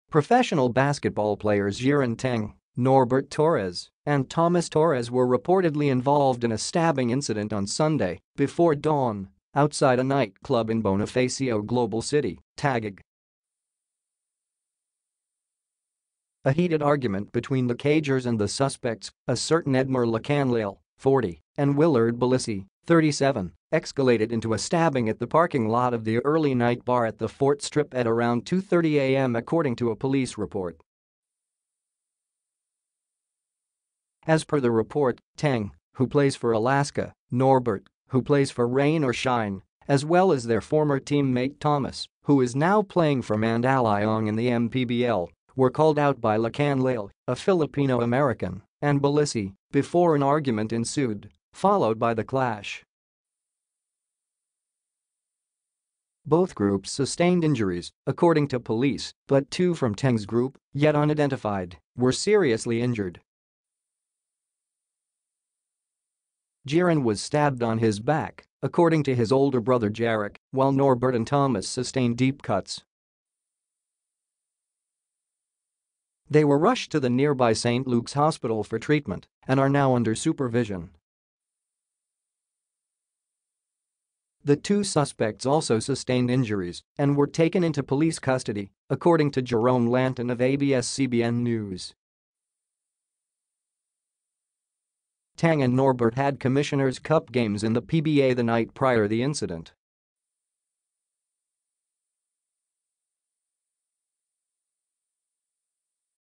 Jeron Teng, 2 other players involved in scuffle
Professional basketball players Jeron Teng, Norbert Torres, and Thomas Torres were reportedly involved in a stabbing incident on Sunday, before dawn, outside a night club in Bonifacio Global City, Taguig.

A heated argument between the cagers and the suspects, a certain Edmar Lacanlale, 40, and Willard Balisi, 37, ecscalated into a stabbing at the parking lot of the Early Night Bar at the Fort Strip at around 2:30 AM according to a police report.

As per the report, Teng, who plays for Alaska, Norbert, who plays for Rain or Shine, as well as their former teammate Thomas, who is now playing for Mandaluyong in the MPBL, were called out by Lacanlale, a Filipino-American, and Balisi, before an argument ensued, followed by the clash.

Both groups sustained injuries, according to police, but two from Teng's group, yet unidentified, were "seriously injured".

Jeron was stabbed on his back, according to his older brother Jeric, while Norbert and Thomas sustained deep cuts.

They were rushed to the nearby St. Luke's Hospital for treatment and are now under supervision.

The two suspects also sustained injuries, and were taken into police custody, according to Jerome Lantin of ABS-CBN News.

Teng and Norbert had Commissioner's Cup games in the PBA the night prior the incident.